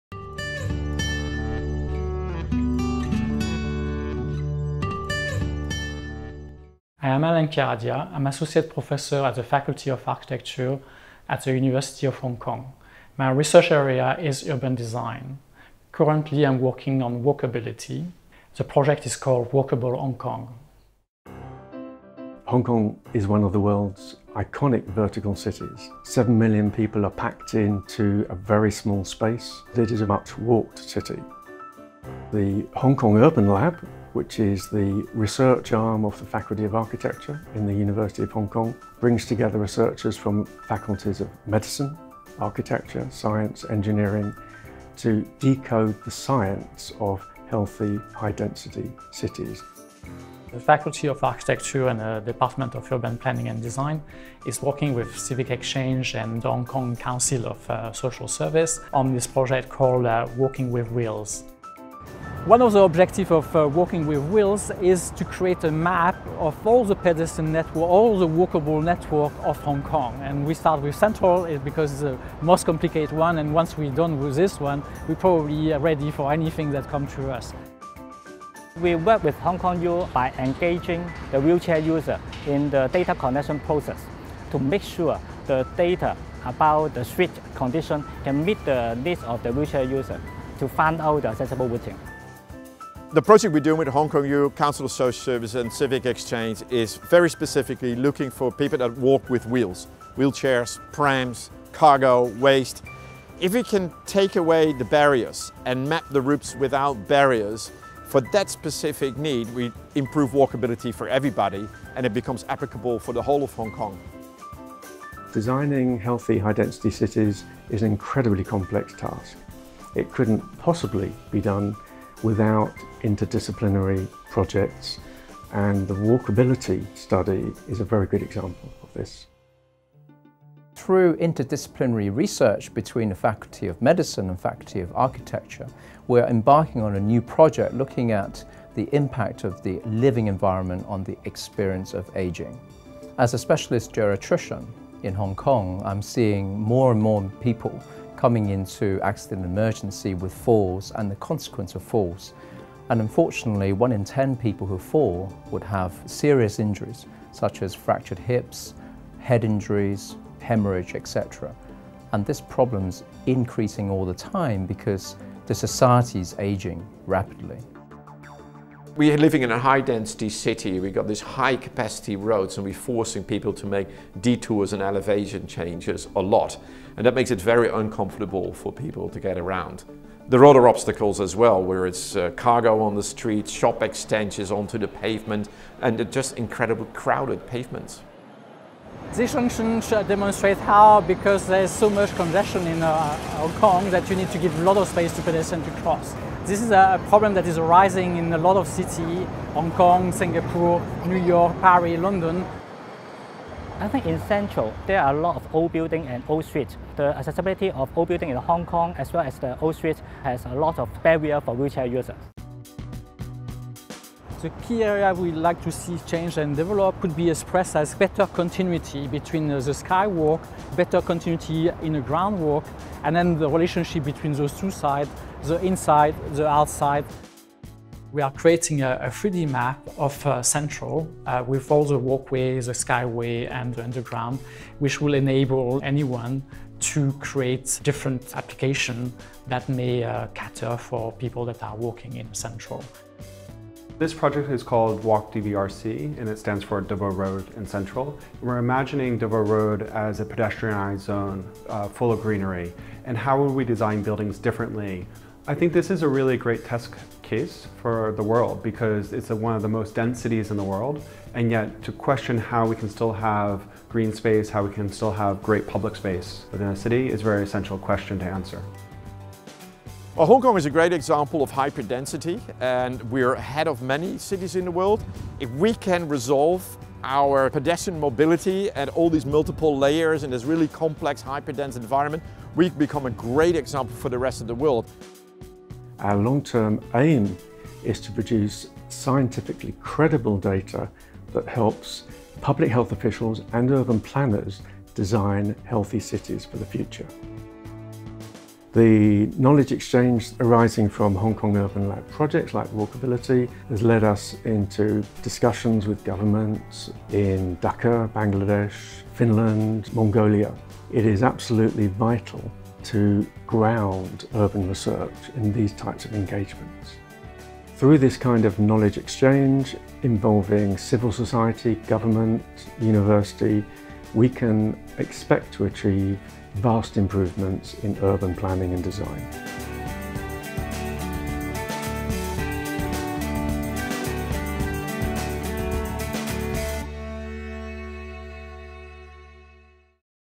讓香港成為更宜步行的城市
香港是世界上其中一個最繁忙和最密集的高空發展城市，但香港亦是全球最適宜步行的城市之一，擁有由交通樞紐，市場，購物中心和住宅區連接的多重行人通道網絡。然而，隨著人口老齡化，而且新的基建項目和高樓層的建築擠入本已十分擁擠的城市空間，令到行人越來越難找到清晰直接的路線通往目的地。香港大學都市實驗室是香港大學建築學院的研究及知識交流單位，現正與思匯政策研究所及香港社會服務聯會合作，開展了一個名為 ‘Walking with Wheels’的項目，目的是為輪椅、嬰兒車和手推車使用者尋找最好的無障礙通道。